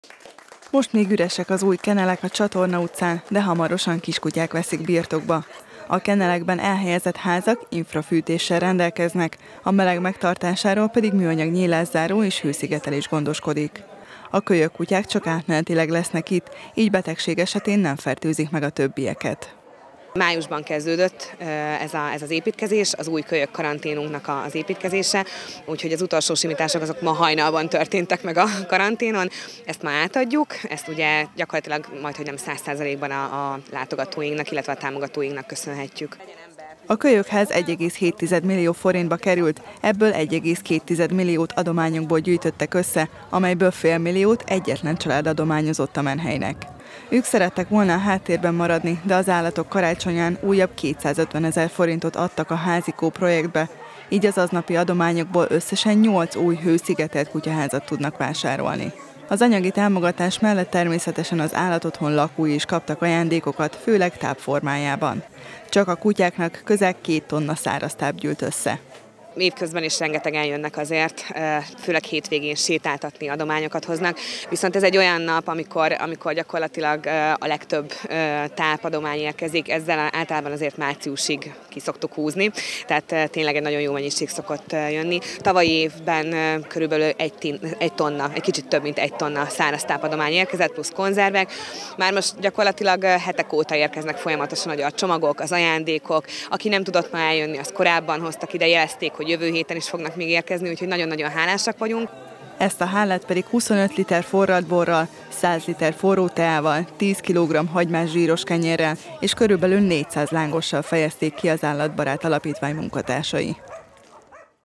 Állatok karácsonya
Közel két millió forintból épült új, nyolc férőhelyes kölyökház az Állatbarát Alapítvány Csatornai utcai menhelyén. Az épületet vasárnap, az Állatok karácsonyán adták át, ahol a látogatók közel két tonna tápdományt tettek az kutyáknak a karácsonyfa alá. Az aznap befolyt összegből 8 hőszigetelt kutyaházat is tudnak vásárolni a menhely dolgozói.